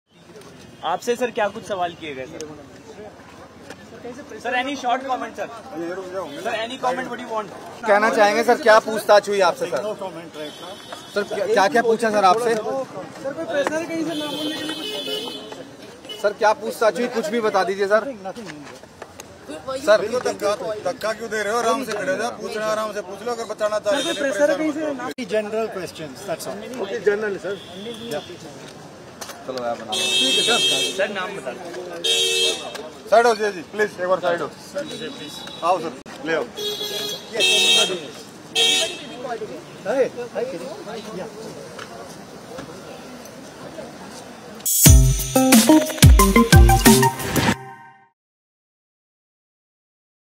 News Portal विवाद मामले में पूछताछ के बाद निकले वरिष्ठ पत्रकार D. Raghunandan सुनिए क्या जवाब दिया?
पूछताछ के बाद उर्मिलेश सेल के दफ्तर से बाहर निकले.इससे पहले अनिंदो भी जा चुके है.दोनों ने कैमरे पर कोई बात नहीं की.अभिसार शर्मा भी निकले.डी रघुनंदन भी निकले